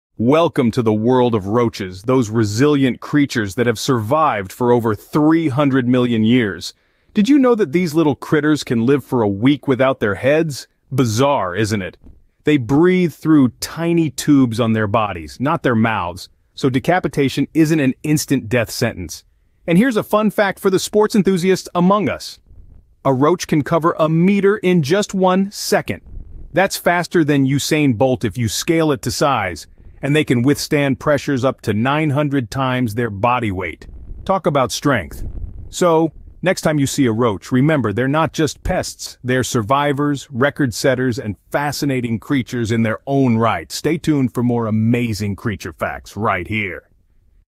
Faster Than Bolt! Roach
Forget everything you think you know about roaches! These creepy crawlies are actually champions of survival. Did you know they can live for a week without their heads? Or run faster than Usain Bolt if you shrink them down? They're tough, adaptable, and full of surprises. So next time you see a roach, remember, they're not just pests – they're fascinating creatures with incredible abilities. Stay curious, explore the unseen, and join us for more mind-blowing animal facts! ➡️